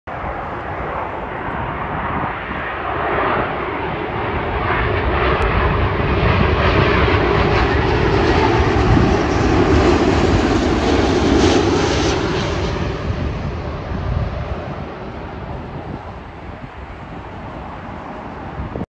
Boeing 777 (777-236ER) | G-YMMJ | British Airways | London Heathrow - LHR | 02/12/21
A BA 777 lands at Heathrow following a flight from Kuwait City.